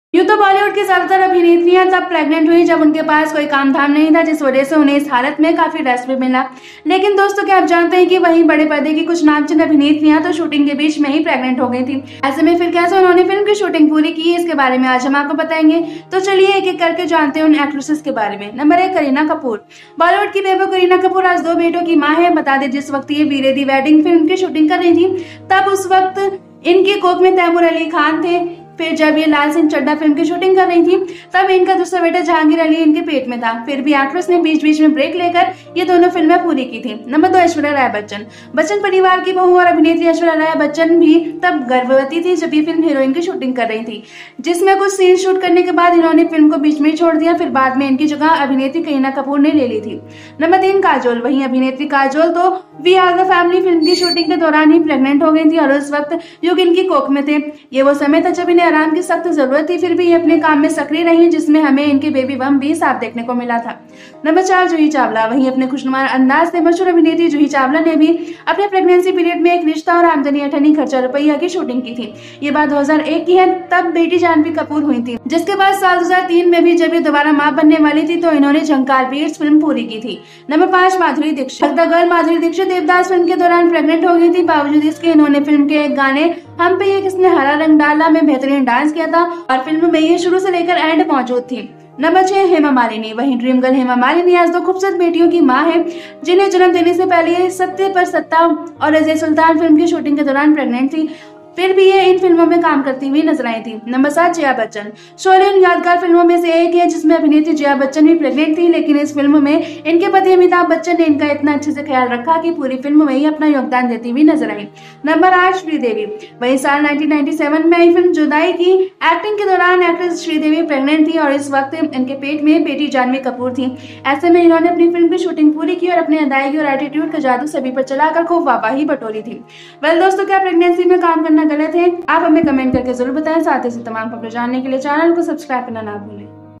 काजोल सहित शूटिंग करते वक्त प्रेग्नेंट हो गई थी बॉलीवुड industry की यह मशहूर एक्ट्रेसेस
bollywood_all_news_biography_bollywood_these_These famous actresses of Bollywood industry became pregnant while shooting including Kajol